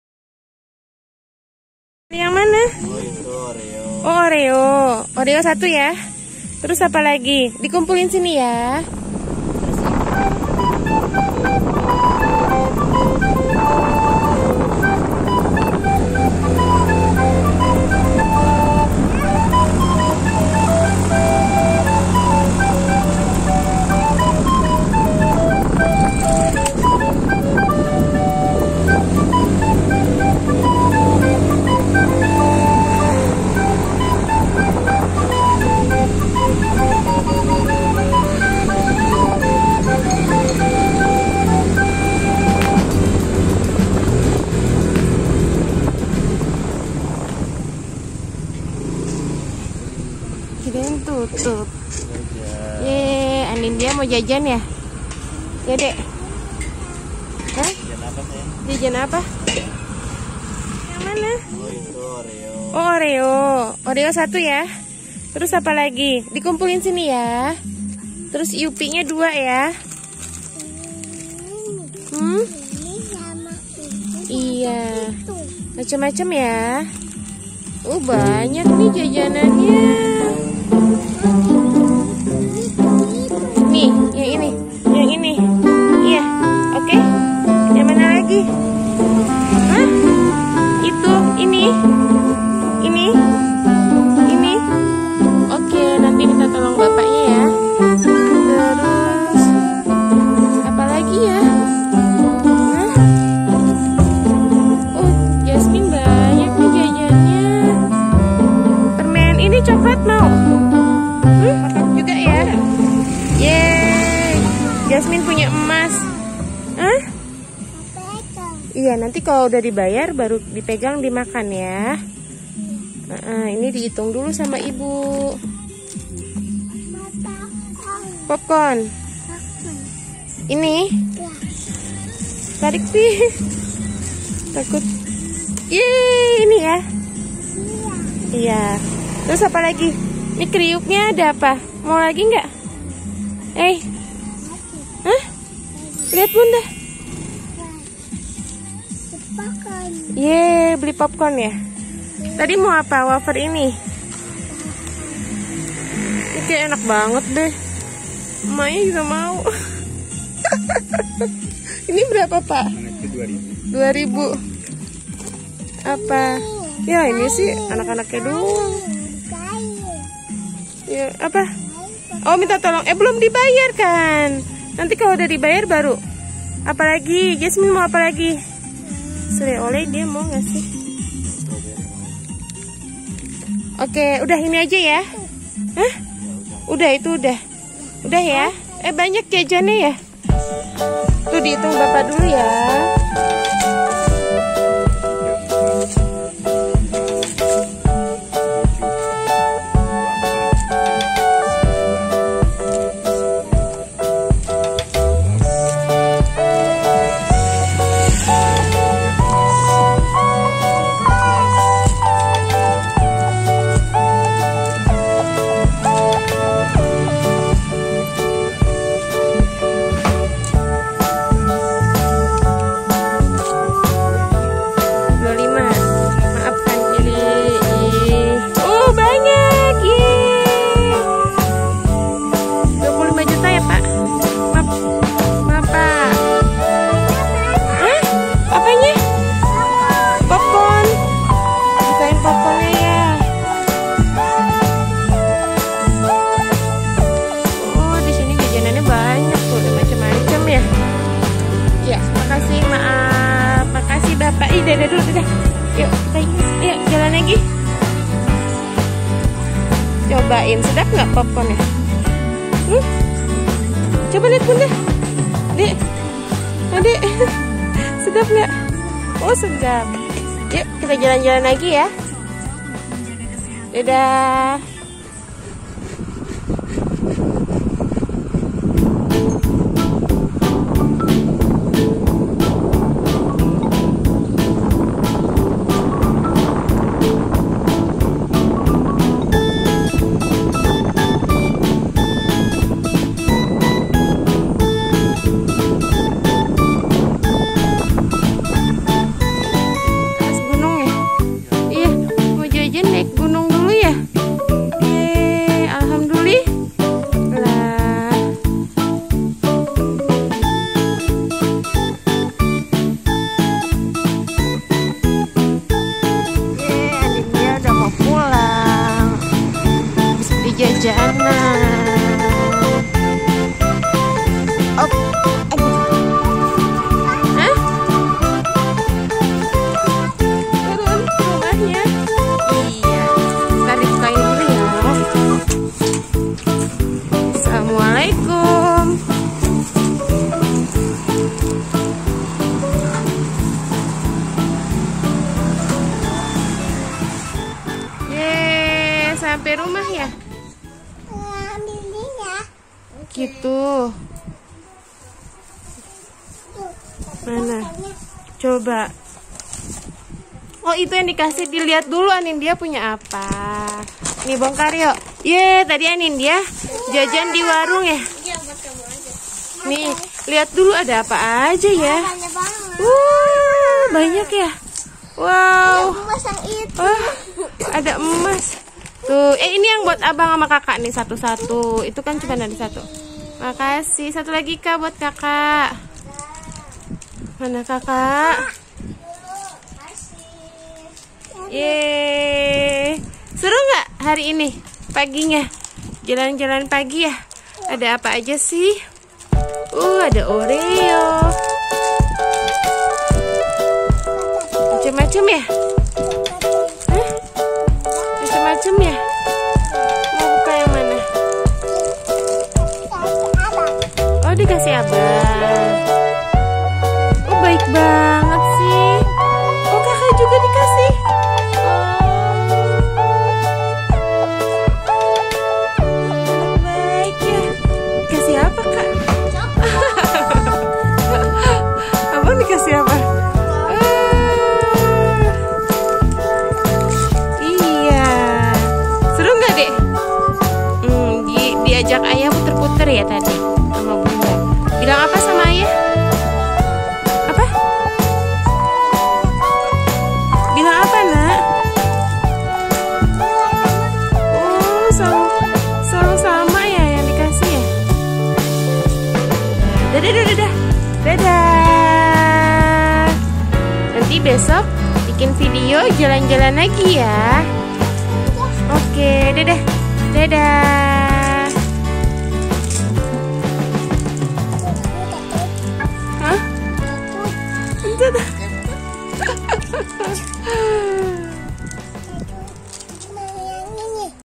Jasmine beli banyak jajanan.. naik motor ke gunung.. seruuu || cerianggu family vlog..
daily activity.. video ini hanya untuk bersenang2 ya.. melihatkan betapa indahnya alam sekitar.. dan jasmine jajan banyak sekali..naik motor ke atas menuju gunung.. seruuu.. happy ya..

cerianggu family vlog..